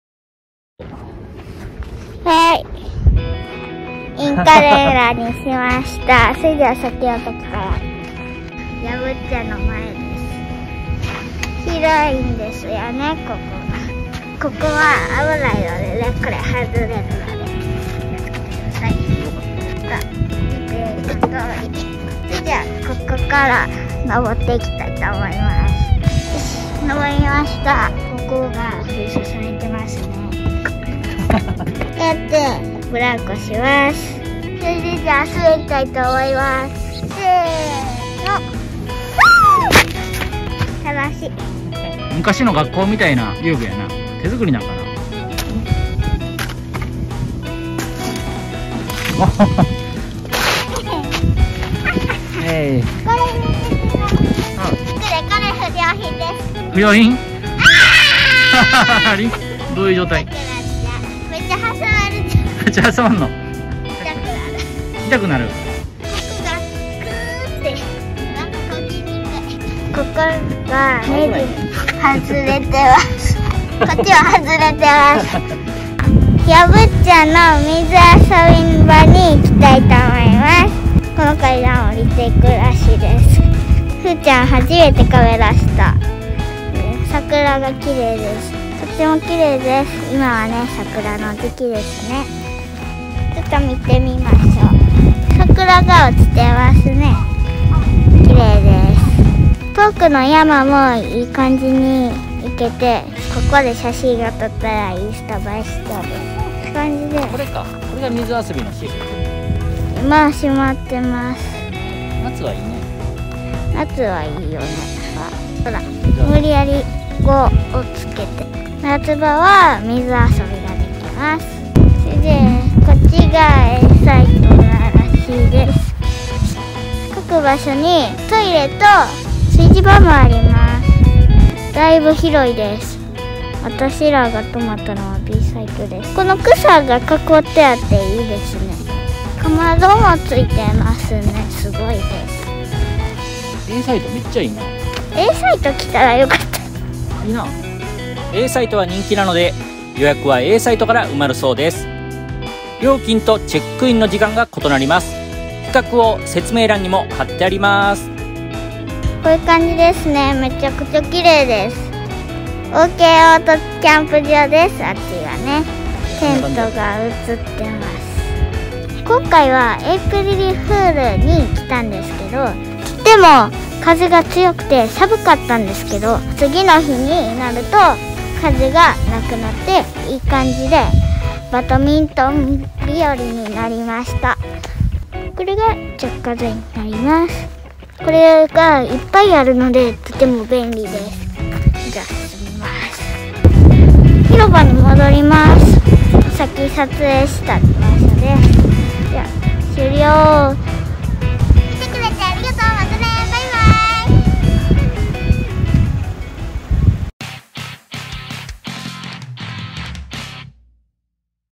ふうちゃん撮影！やぶっちゃ★キャンプ実況！🗺Aサイトと遊具！⛺️
ふうちゃんが初めてカメラを持って、実況しながら撮影をしました！😊

AサイトとBサイトの比較です！

Aサイトの方が広くて便利だし良いんですが、チェックインが午後2時になってます。

Bサイトは狭いですが、チェックインは12時です。

Aサイト
チェックイン　午後2時
チェックアウト　午前11時
サイト使用料 5500円
衛生管理費 110円
電気使用量 1100円
電気も入れると 一泊6710円

Bサイトチェックイン　正午12時チェックアウト　午前11時
サイト使用料 4700円
電気も入れると 一泊5800円

デイキャンプ
午前9時〜午後5時
大人1人 700円
子供 1人 400円